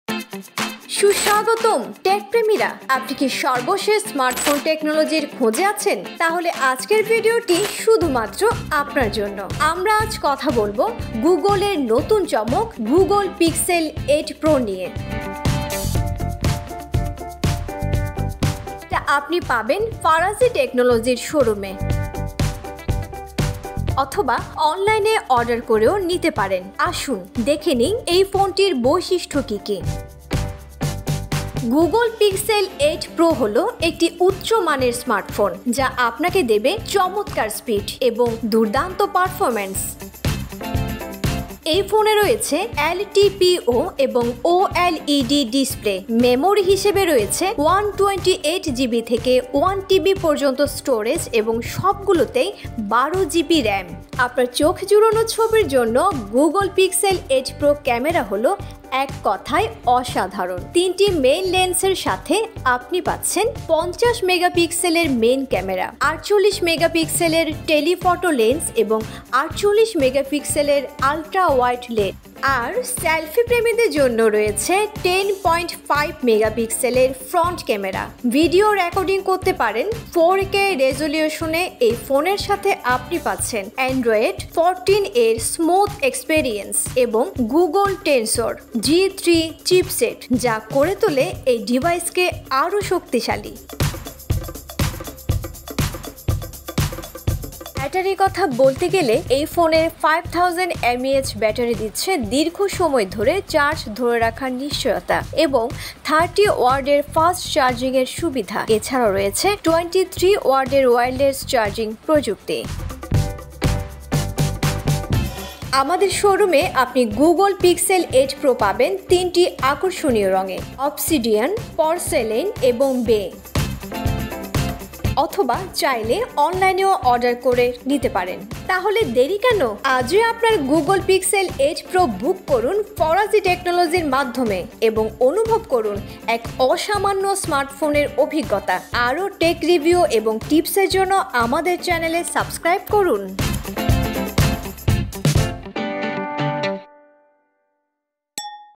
Google Pixel 8 Pro Price in Bangladesh | ফারাজী টেকনোলজি | Bangla Review
"Google Pixel 8 Pro রিভিউ: ফারাজী টেকনোলজি আপনাদের জন্য নিয়ে এসেছে, গুগলের নতুন স্মার্টফোনের বিস্তারিত পর্যালোচনা। দেখে নিন এর অসাধারণ ডিজাইন, উন্নত ক্যামেরা সিস্টেম, শক্তিশালী পারফরম্যান্স এবং আরও অনেক কিছু।"

১. প্রশ্ন: Google Pixel 8 Pro ফোনটির বাজারে আসার তারিখ কি?
উত্তর: Google Pixel 8 Pro ঘোষণা করা হয় ২০২৩ সালের অক্টোবর ৪ তারিখে এবং বাজারে আসে ২০২৩ সালের অক্টোবর ১২ তারিখে।

২. প্রশ্ন: Google Pixel 8 Pro ফোনের ওজন কত?
উত্তর: Google Pixel 8 Pro ফোনের ওজন হল ২১৩ গ্রাম।

৩. প্রশ্ন: Google Pixel 8 Pro-এর ডিসপ্লের বৈশিষ্ট্য কী কী?
উত্তর: এর ডিসপ্লে হল LTPO OLED, ১২০Hz রিফ্রেশ রেট, HDR10+ সাপোর্ট এবং ১৬০০ নিটস পর্যন্ত হাই-ব্রাইটনেস।

৪. প্রশ্ন: Google Pixel 8 Pro ফোনের ক্যামেরা কনফিগারেশন কেমন?
উত্তর: এতে রয়েছে তিনটি মূল ক্যামেরা - ৫০ মেগাপিক্সেল ওয়াইড, ৪৮ মেগাপিক্সেল টেলিফটো এবং ৪৮ মেগাপিক্সেল আলট্রাওয়াইড।

৫. প্রশ্ন: বাংলাদেশে Google Pixel 8 Pro ফোনটির দাম কত?
উত্তর: বাংলাদেশে Google Pixel 8 Pro ফোনের দাম বাজারভেদে ভিন্ন হতে পারে, তবে আন্তর্জাতিক মূল্য হল $ 979.99 / € 1,098.99 / £ 877.75।

➤ Farazi Technology

Level 4, Shop No: 003D
Block-C. Jamuna Future Park, Dhaka.